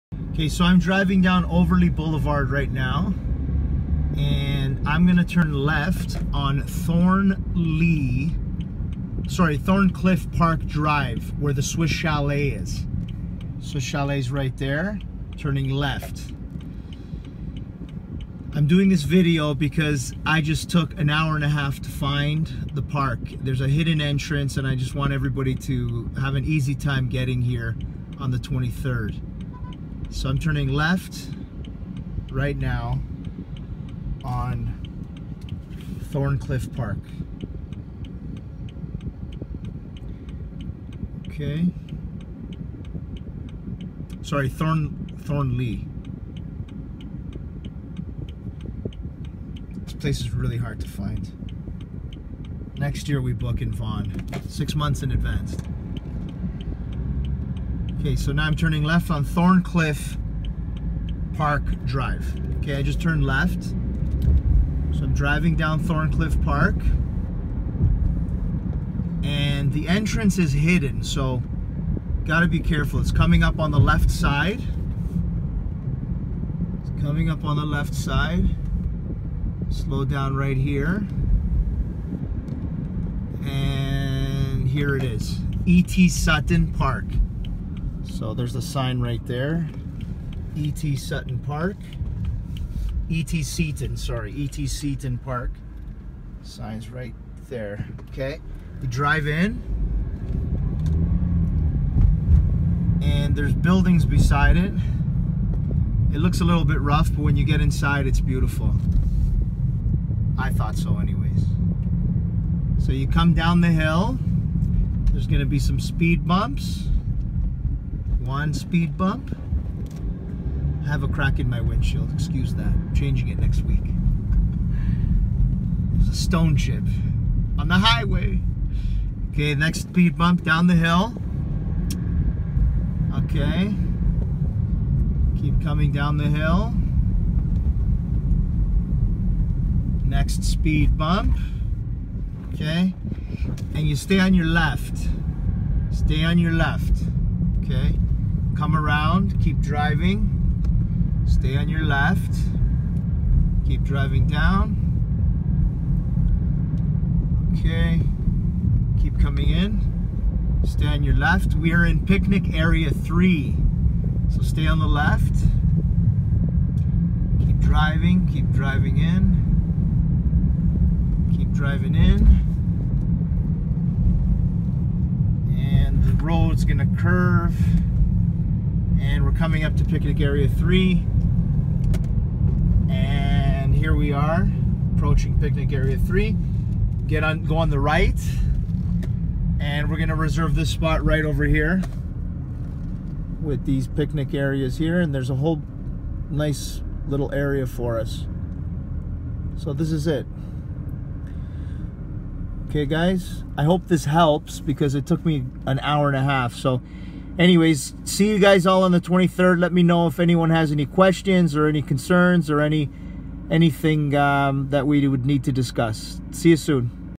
Directions to ET Seton Park
Drive to ET Seton Park  from Overlea Blvd. turning left on Thorncliffe Park Drive and then left into the ET Seton Entrance.  Keeping left all the way to picnic area #3...